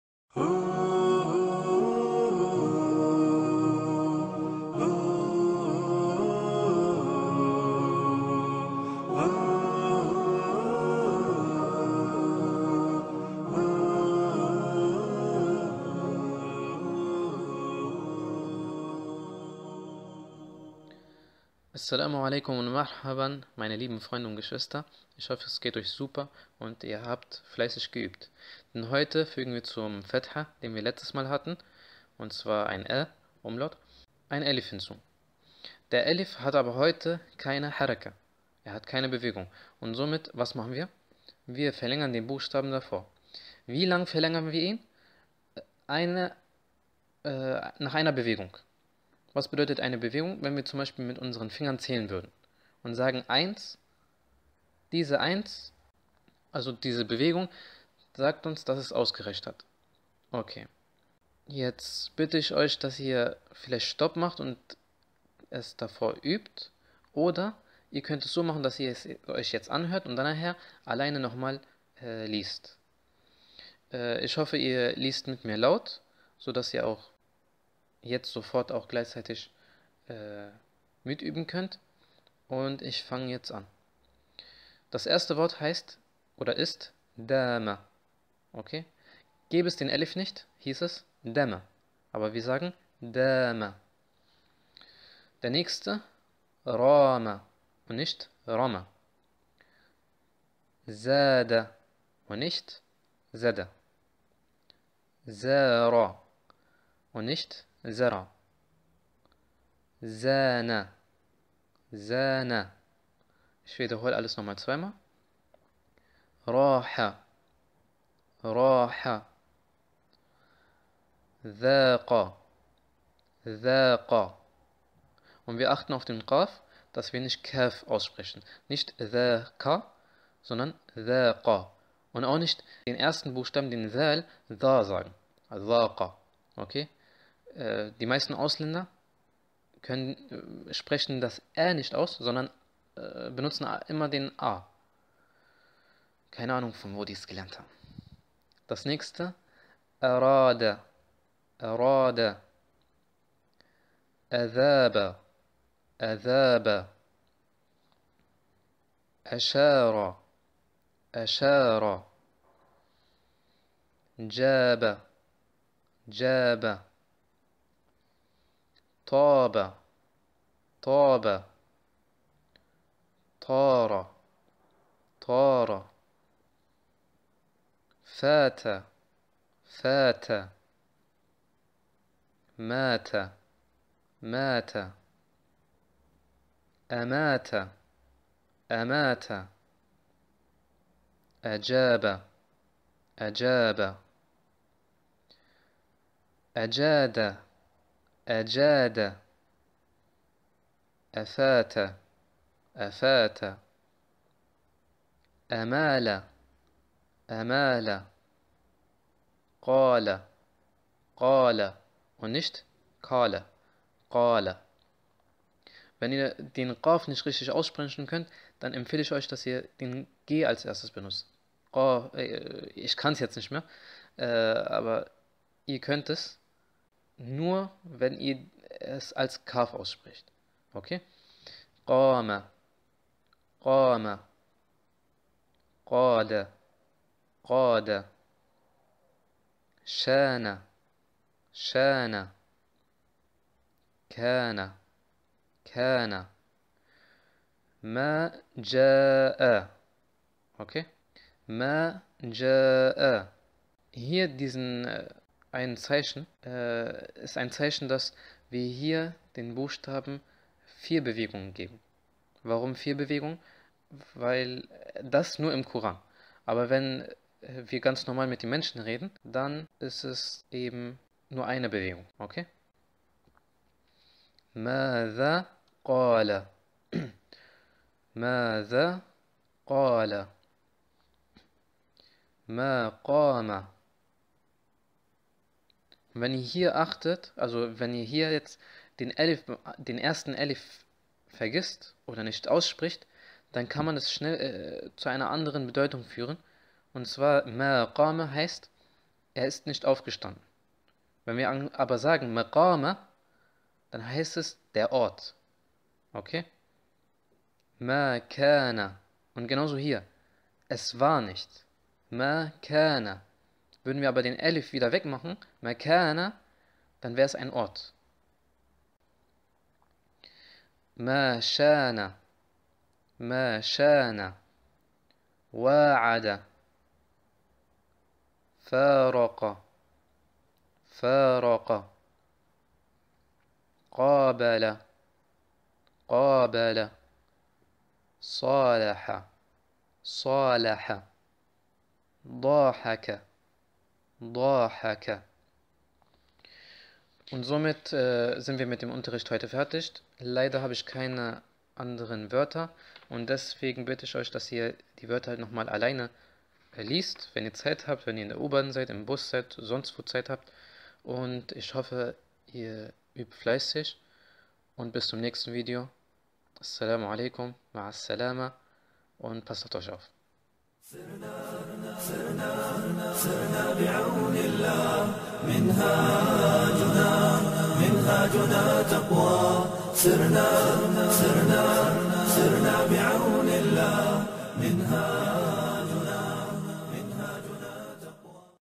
Bilkalem Die arabischen Buchstaben A1 Lektion 06
Hier findet ihr die Hefte, um das Schreiben der arabischen Buchstaben zu lernen

In diesen Serien lernt ihr, wie man die arabischen Buchstaben schreibt und liest, ebenso was die Haraka sind, die Buchstaben, die zum verlängern dienen und wie man dann Wörter und ganze Sätze liest. Ich habe es wird euch Spaß machen und vergisst nicht mir ein Like zu geben und ein Kommentar zu lassen.

Bilkalem